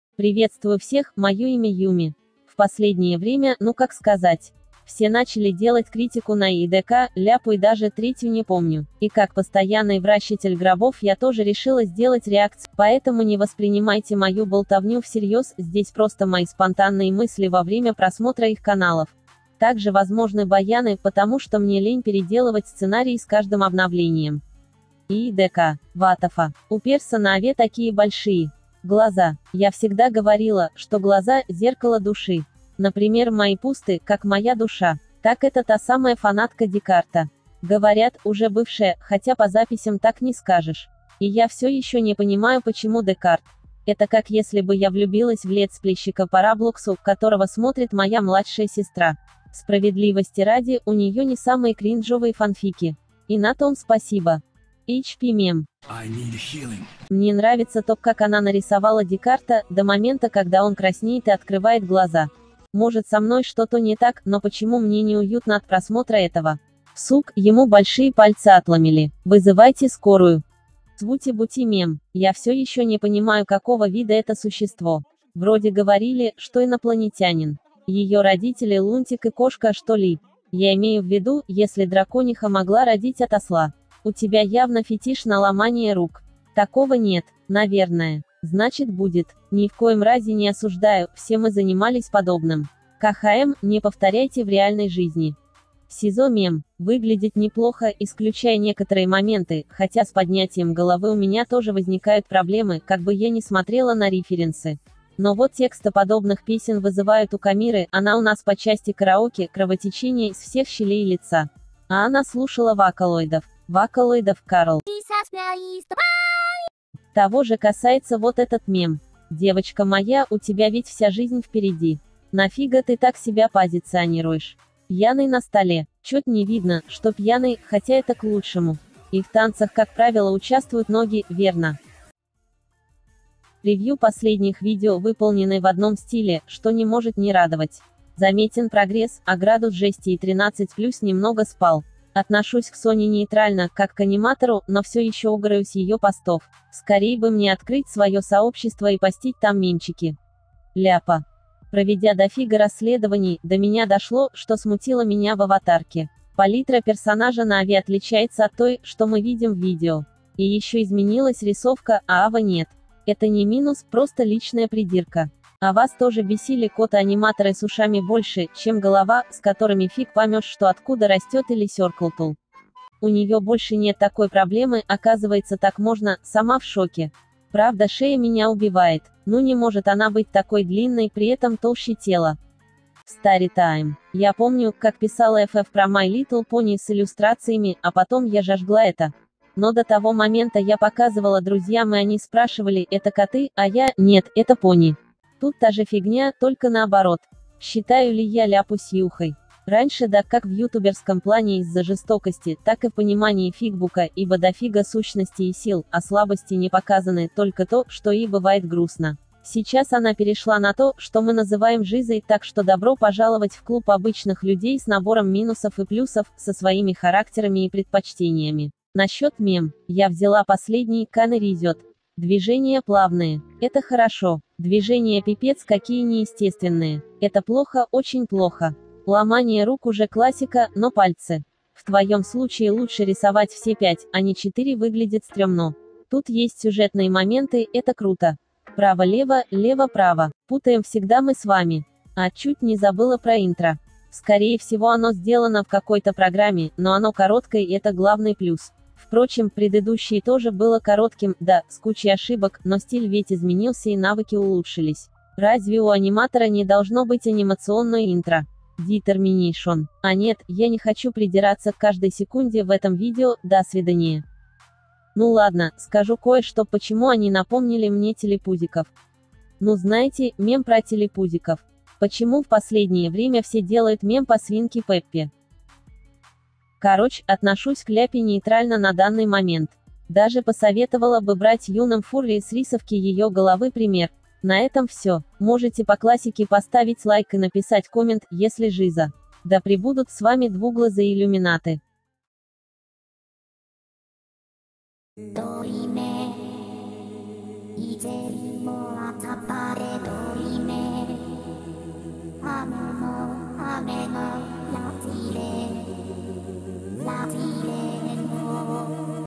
Мысли вслух: И.и.Д.К., Ляпа и... я забыла (ч.о.)
Внимание: СУБЪЕКТИЗМ!

Данное видео является чем-то средним между обзором и реакцией. Это НЕ разбор накрутки или что-то вроде.
Я озвучиваю свои мысли: что заметила, на что закрыла глаза, что мне напомнил тот или иной фрагмент – и в конце собираю мысли в кучку, объединяя их в общий вердикт.

Если данный формат видео вам понравился, можете написать в комментариях, кого ещё мне стоит оценить. Тот, кто угадает забытую персону на превью – попадёт в следующий ролик.